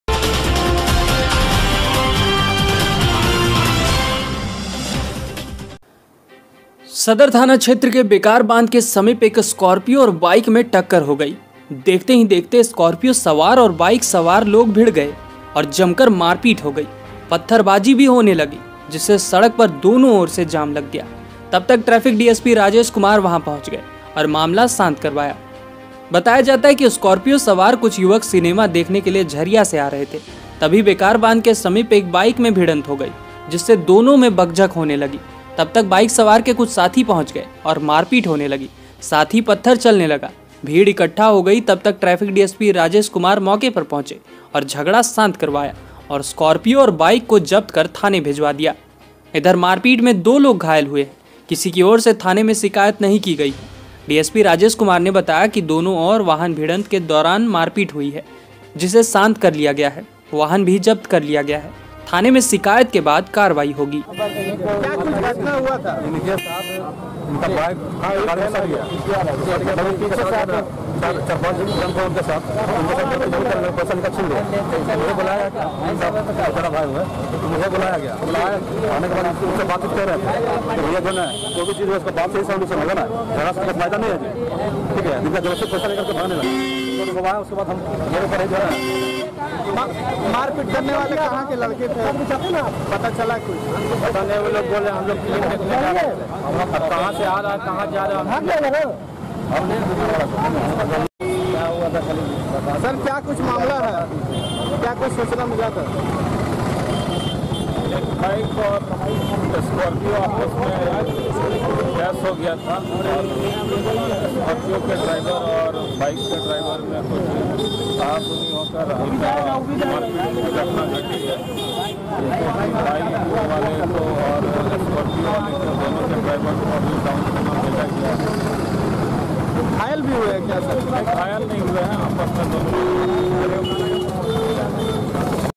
स्कॉर्पियो और बाइक में टक्कर के बाद मामला पकड़ा तूल,मौके पर पहुँचे ट्रैफिक डी एस पी राजेश कुमार
Dhanbad: सदर थाना क्षेत्र के बेकारबांध के समीप एक स्कॉर्पियो और बाइक में टक्कर हो गई देखते ही देखते स्कॉर्पियो सवार और बाइक सवार लोग भिड़ गए और जमकर मारपीट हो गई. पत्थरबाजी भी होने लगी. जिससे सड़क पर दोनो ओर से जाम लग गया. तब तक ट्रैफिक डीएसपी राजेश कुमार वहा पंहूच गए और मामला शांत करवाया.

बताया जाता है की स्कॉर्पियो सवार कुछ युवक सिनेमा देखने के लिए झरिया से आ रहे थे तभी बेकरबांध के समीप एक बाइक में भिड़ंत हो गई जिससे दोनो में बकझक होने लगी तब तक बाइक सवार के कुछ साथी पंहुच गए और मारपीट होने लगी साथ ही पत्थर चलने लगा. भिड़ इक्ट्ठा हो गई तब तक ट्रैफिक डीएसपी राजेश कुमार मौके पर पहुंचे और झगड़ा शांत करवाया.और स्कॉर्पियो और बाइक को जप्त कर थाना भेजवा दिया.

इधर मारपीट में दो लोग घायल हुए हैं. किसी की ओर से थाना में शिकायत नही की गई हैं.डीएसपी राजेश कुमार ने बताया की दोनो और वाहन भिड़ंत के बात मारपीट हुई हैं ही जिसे शांत कर लिया गया है.वाहन भी जप्त कर लिया गया है थाने में शिकायत के बाद कार्रवाई होगी.